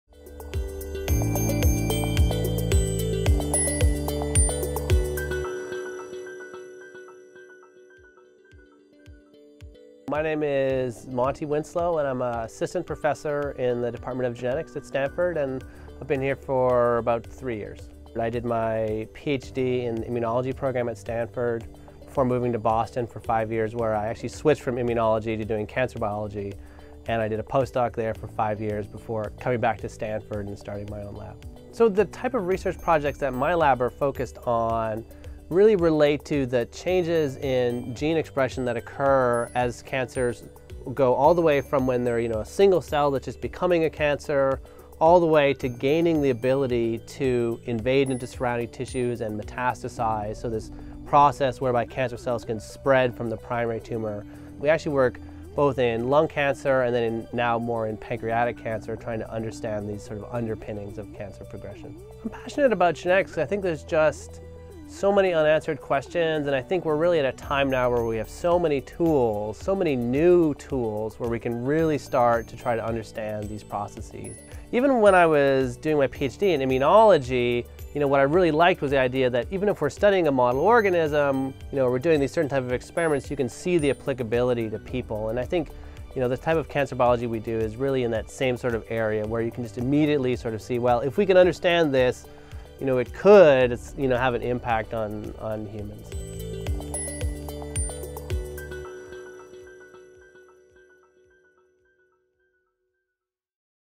Stanford Faculty - Meet Monte Winslow
Monte Winslow, Assistant Professor, Genetics and of Pathology at Stanford University.

Member, Bio-X
Member, Stanford Cancer Institute

Honors & Awards

Scholar Award, The V Foundation for Cancer Research (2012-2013)
Scholar Award, Baxter Foundation (2011)
Genentech Postdoctoral Fellowship, Massachusetts Institute of Technology (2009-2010)
Damon Runyon Cancer Research Foundation, Fellowship (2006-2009)
Hugh McDevitt Prize in Immunology, Stanford University (2006)
Pre-Doctoral Fellowship, Howard Hughes Medical Institute (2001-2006)
Stanford University Graduate Fellowship, Stanford University (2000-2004)